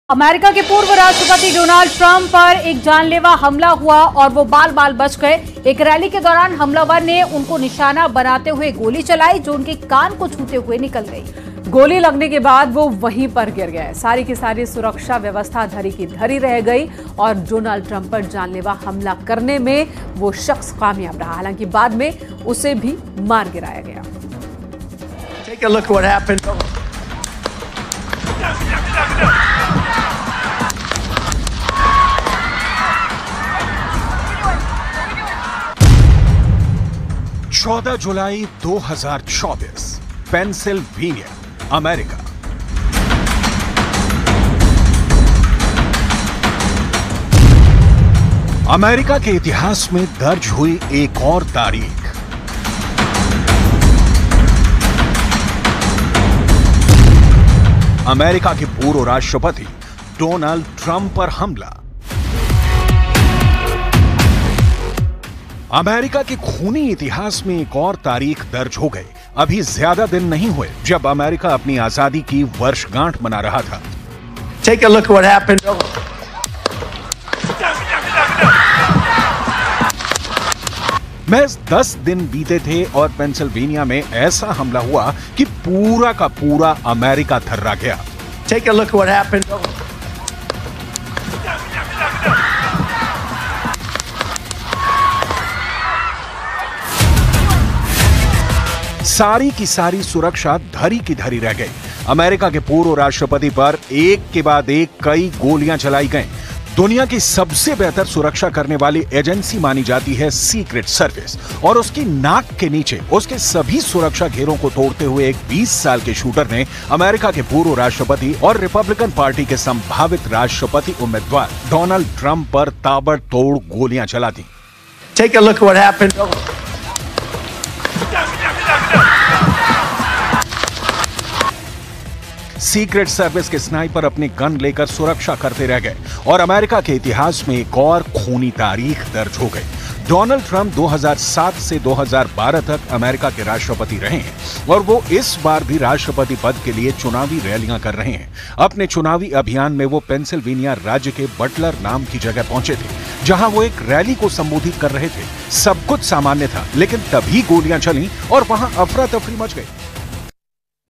Attack on Donald Trump: किसने रची ट्रंप को मारने की साजिश?, हो गया बड़ा खुलासा| America |Biden |N18G
पूर्व अमेरिकी राष्ट्रपति डोनाल्ड ट्रंप का शनिवार को पेंसिल्वेनिया में एक चुनावी रैली थी. इसी दौरान उनकी रैली में गोली चलने की आवाजें सुनाई दी. रैली में संभावित गोलियों की आवाज जैसी तेज आवाजें सुनाई देने के बाद सीक्रेट सर्विस एजेंटों द्वारा उन्हें सुरक्षात्मक तरीके से मंच से उतारा गया.